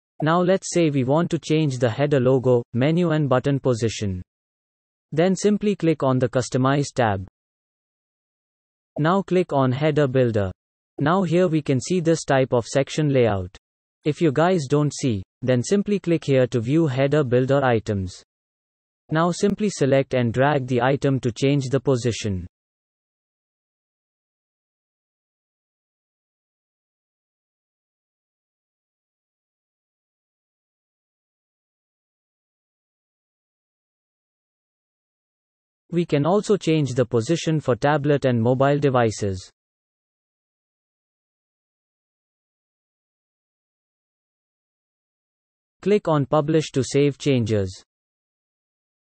Change Position for Header Logo, Menu, Button (WordPress Website Astra Theme)
Change Position for Header Logo Menu, Button (WordPress Website Astra Theme) @HowToMakeAWebsiteForBeginners